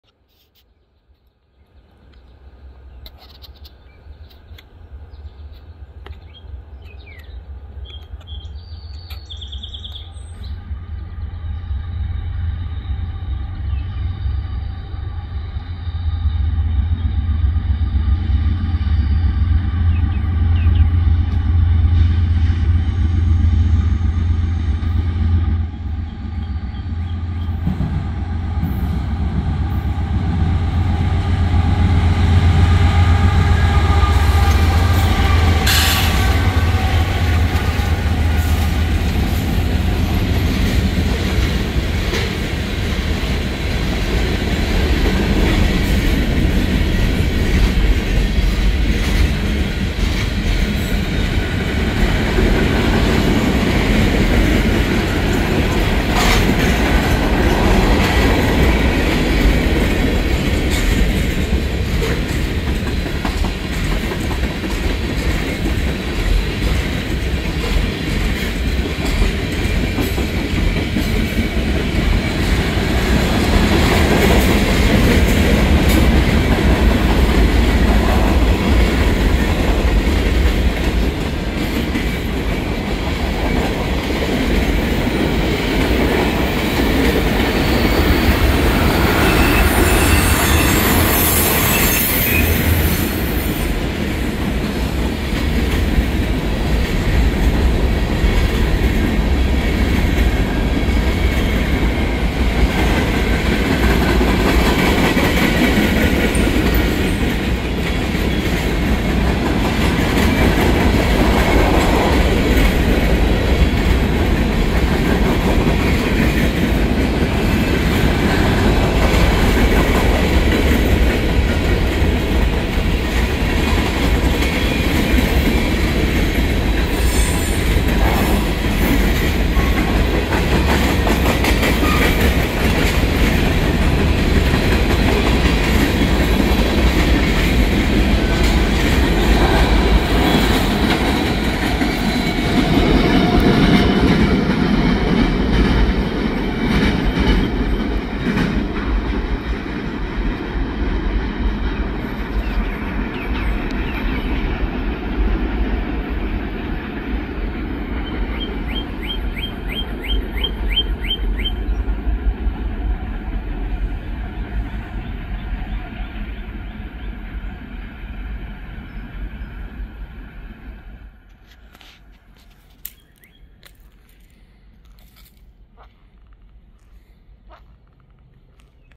Norfolk Southern #4157 leads a 76 car manifest at Lewistown, PA (07/07/2024)
11K